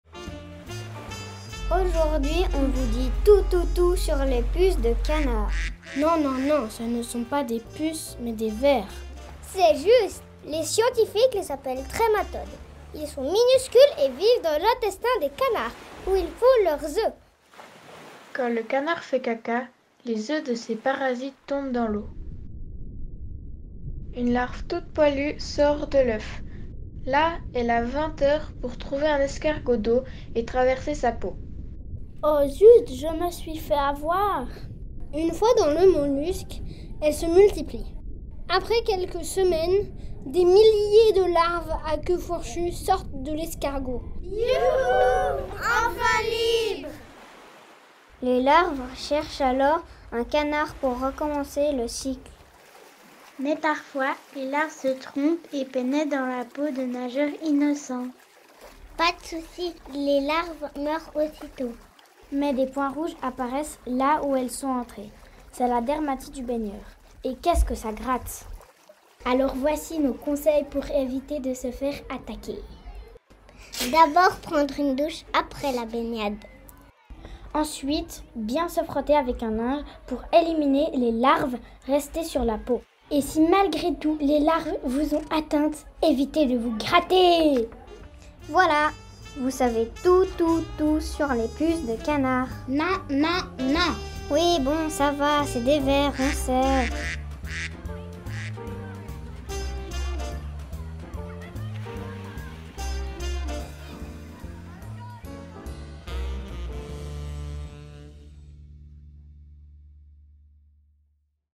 Les Puces de Canard
Film en stop motion réalisé par 9 enfants de 7 à 11 ans lors d’un atelier organisé par l’Atelier des musées le 8 janvier 2022 en collaboration avec Lilo Wullschleger, ethnologue, formatrice et réalisatrice.
Les enfants ont illustré le cycle des puces de canards en lien avec l’exposition A l'eau présentée au Muséum.